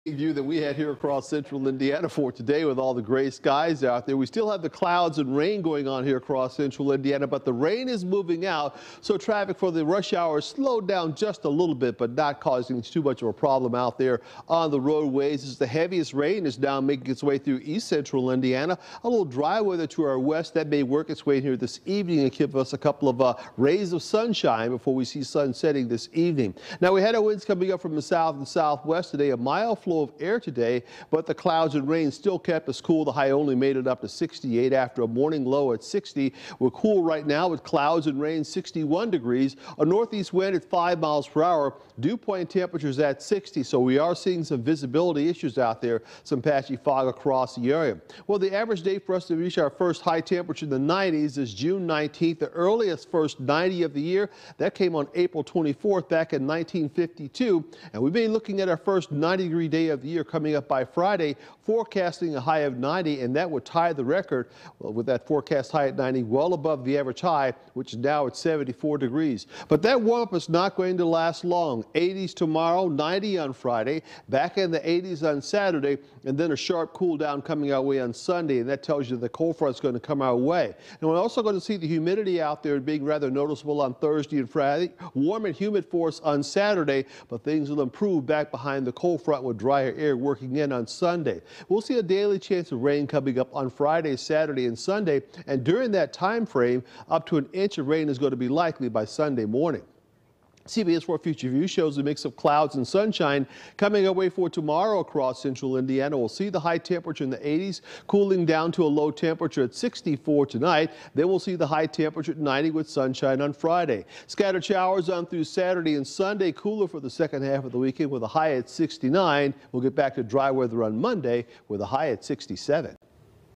Chris forecasts our first 90° day of the year
A record high is possible Friday before rain moves in cools us down.